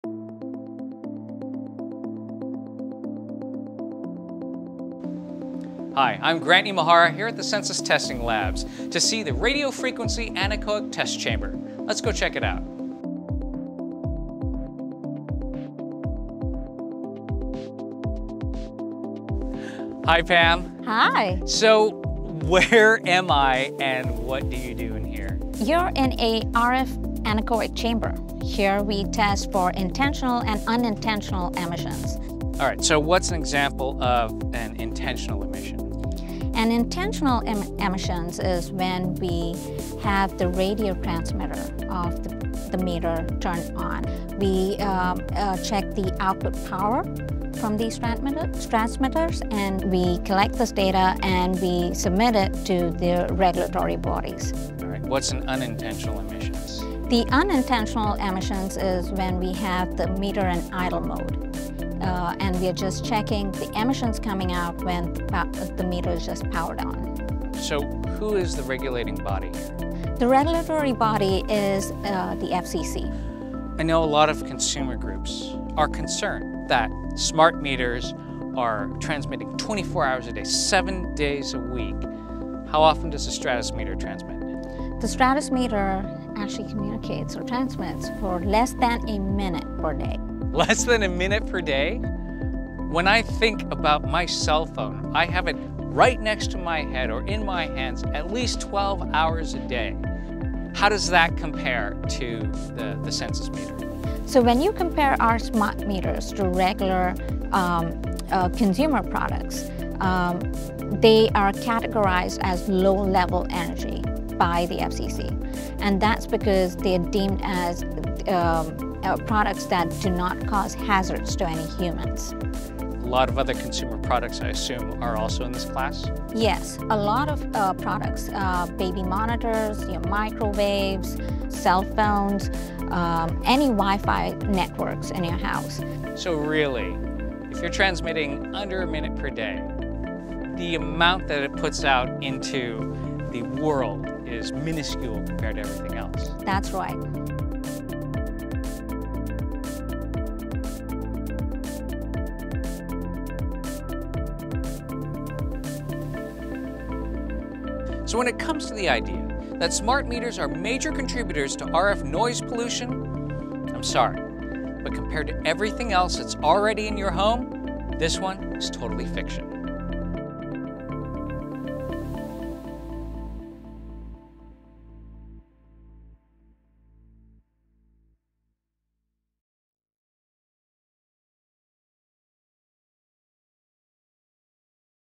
Fact or Fiction: Smart Meter RF Emissions
Grant Imahara visits the Sensus anechoic chamber to understand the minimal levels of RF transmissions by smart meters.